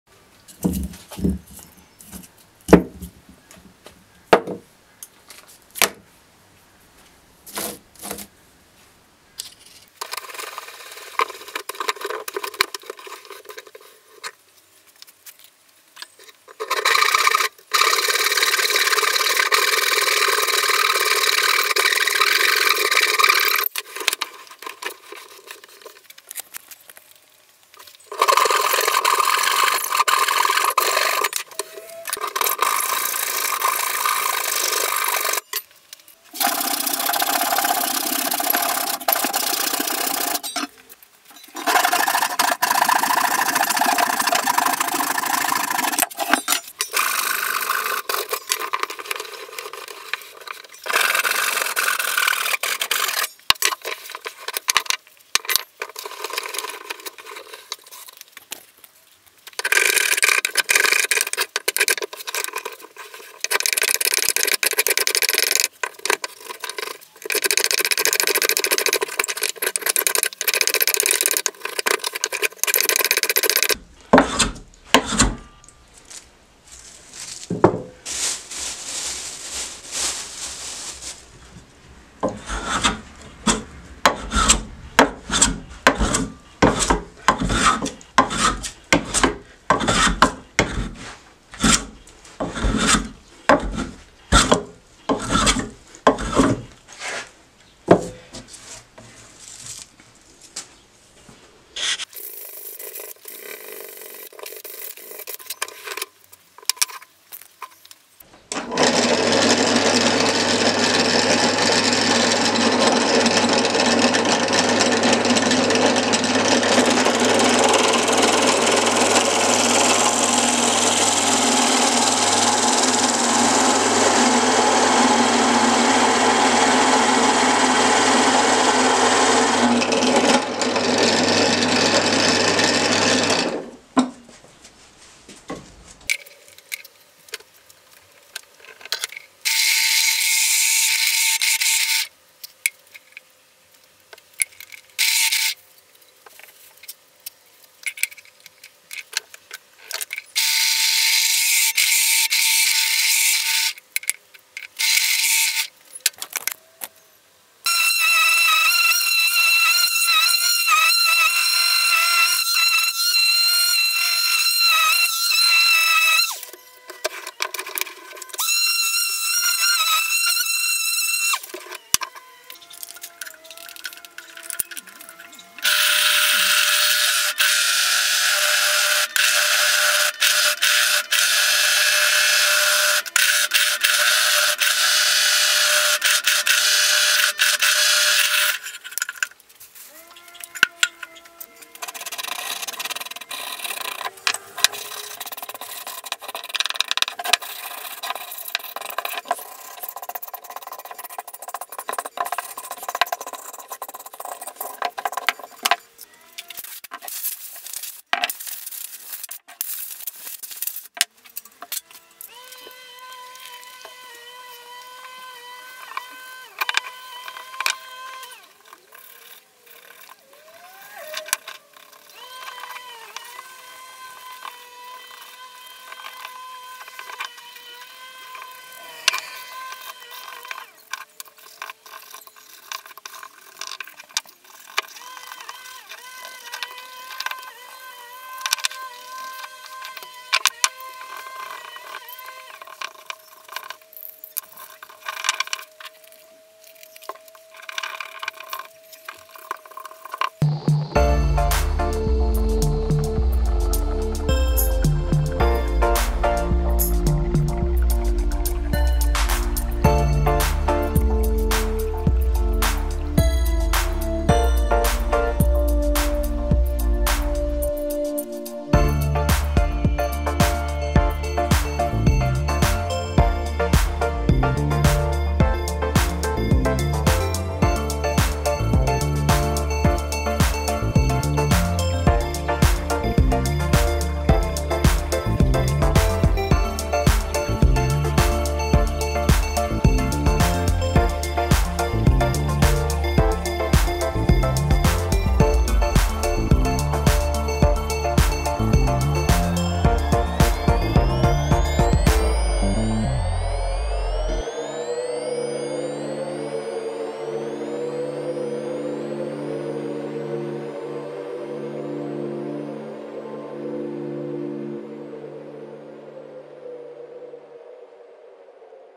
Cherry live edge candle holder ornaments - easy diy
Made some candle holders out of a cherry piece of wood. I love how they turned out, do you?